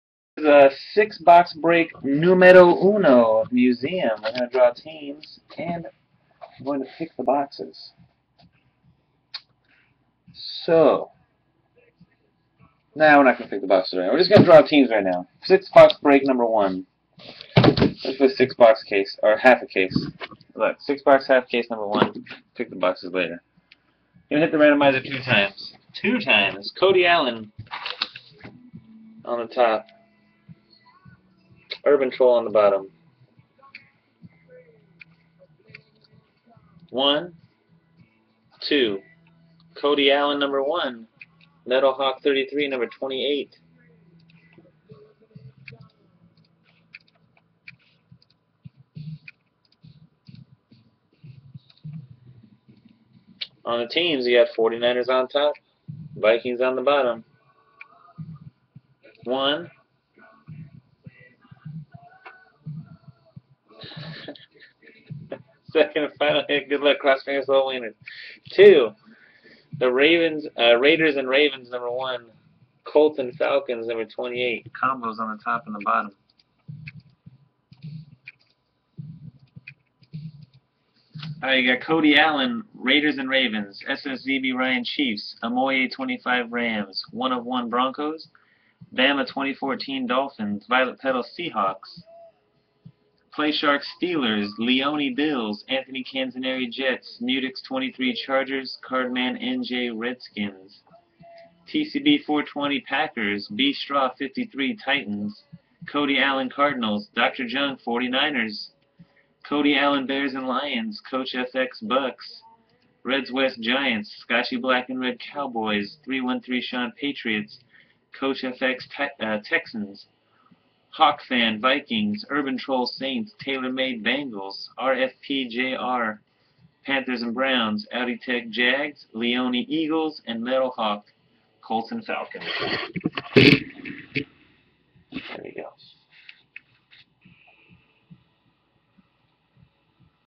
Team Draw Results ~ 2013 Museum Football 6 Box Break #1
1. kodialin     1. raiders/ravens
2. sszbryan     2. chiefs
3. amoye25     3. rams
4. 1of1     4. broncos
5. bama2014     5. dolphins
6. violetpetals38     6. seahawks
7. playshark1     7. steelers
8. maleone41     8. bills
9. anthonycanzoneri     9. jets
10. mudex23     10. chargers
11. Cardmannj     11. redskins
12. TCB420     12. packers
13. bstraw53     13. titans
14. kodialin     14. cardinals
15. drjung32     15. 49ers
16. kodialin     16. bears/lions
17. coachfx     17. bucs
18. redzwest     18. giants
19. ScotchyBlackNRed     19. cowboys
20. 313shawn     20. patriots
21. coachfx     21. texans
22. Hawkfan     22. vikings
23. urbantroll     23. saints
24. TaylorMade     24. bengals
25. RFPJR     25. panthers/browns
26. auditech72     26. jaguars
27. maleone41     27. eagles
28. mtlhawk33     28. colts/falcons